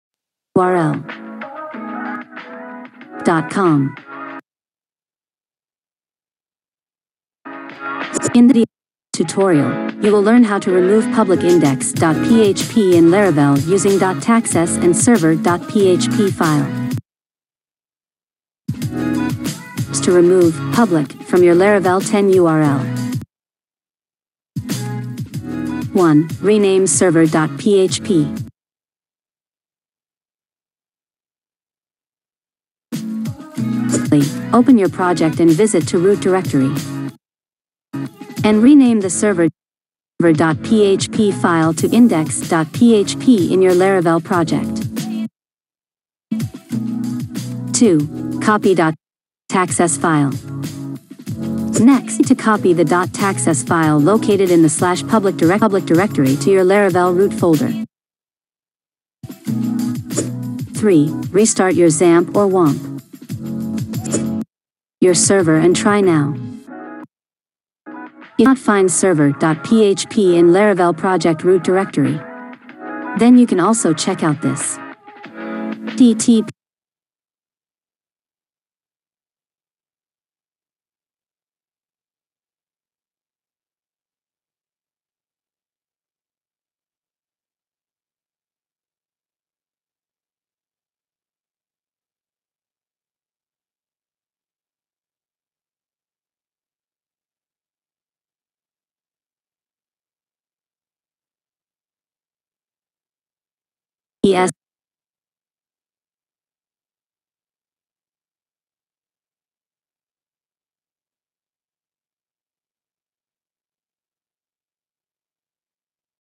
Laravel 10 Remove Public from URL
In this video tutorial, you will learn how to remove public index.php in laravel using .htaccess and server.php file.

Step 1: Rename the server.php file
Step 2: Copy the .htaccess file
Step 3: Restart your Xampp or Wamp

how to remove public from url in laravel 10
Laravel's .htaccess to remove "public" from URL